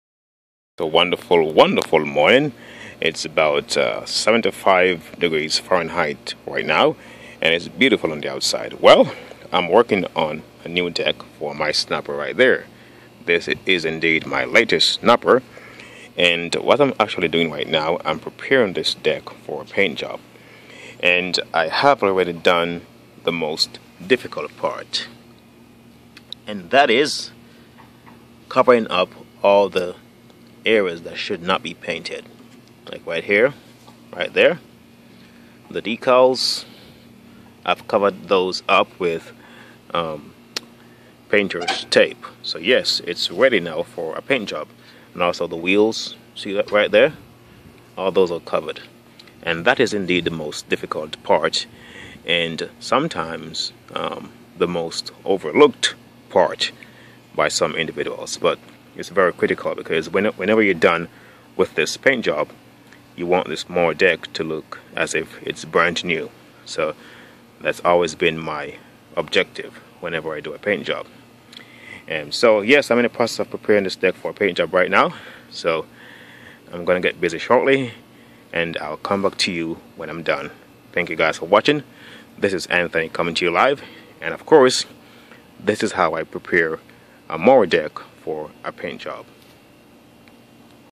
How To: Prepare A Mower Deck For A Paint Job
In this video, I show the most critical step in prepping a mower deck for a repaint job. It's important to take the time and do it right, if you want a professional finish.